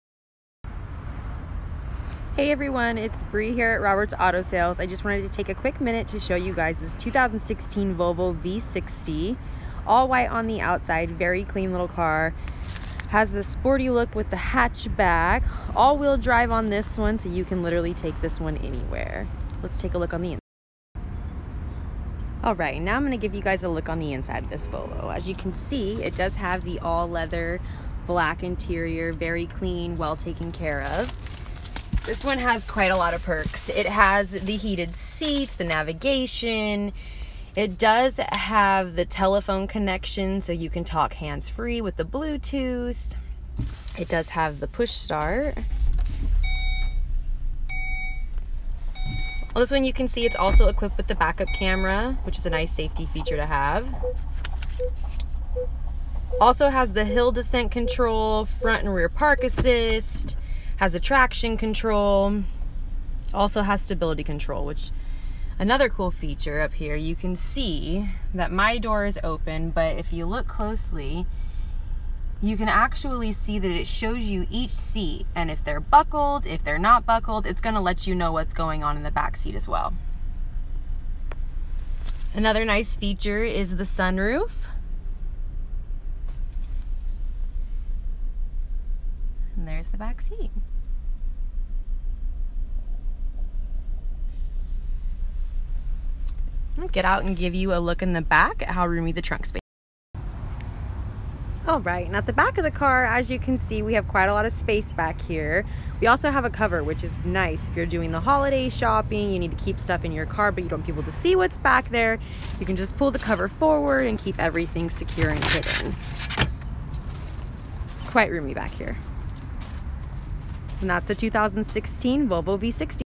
2016 Volvo V60 AWD - Roberts Auto Sales
Brie walks around and gives a brief tour of a 2016 Volvo V60 AWD.  robertsautosales.com